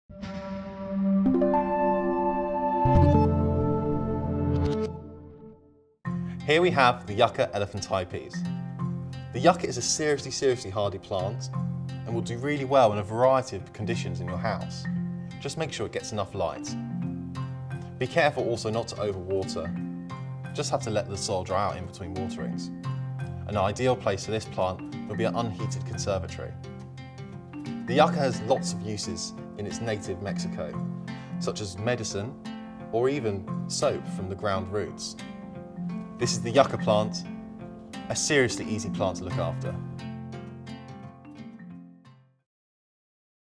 Yucca Plant Care Tips 🌿 How To Care For Yucca elephantipes
Yucca is a distinctive easy-care houseplant. Also known as Yucca elephantipes or Spinelss Yucca, this is a specimen plant used for the architectural value of its leaf clusters, sprouting from upright or branched trunks of varying height.  It is suitable for bright sunny indoor areas.  Indoor Yuccas are produced by lopping off stem sections from mature specimens.  Once rooted, clusters of long, leathery leaves sprout from buds hidden in these truncated stems.

Learn how to care for Yucca elephantipes in this video.

This plant is available at foli8 for UK delivery 🌿

VIDEO TRANSCRIPT:
Here we have Yucca elephantipes. The Yucca is a seriously seriously hardy plant and will do really well in a variety of conditions in your house.  Just make sure it gets enough light, be careful also not to over water, just have to let the soil dry out in between waterings.  An ideal place for this plant will be an unheated conservatory.  The Yucca has lots of uses in its native Mexico such as medicine or even soap from the ground roots.  This is the yucca plant and it is a seriously easy plant to look after.